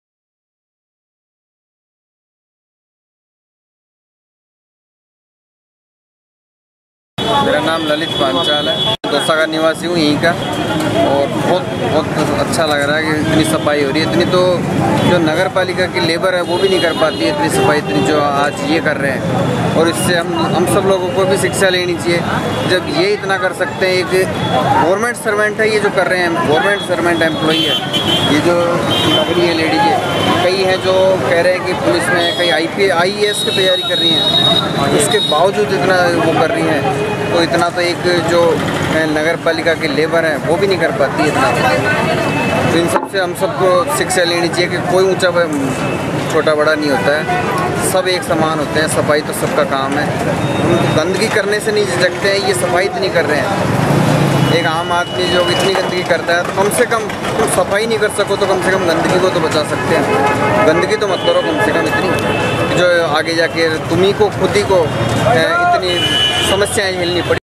Mr. Lalit Kumar, local resident Dausa, sharing his views about cleanliness campaign in Dausa(Raj)
Mr. Lalit Kumar, local resident Dausa, sharing his views about cleanliness campaign organized by Dera Sacha Sauda on 9th October 2013 in Dausa(Raj).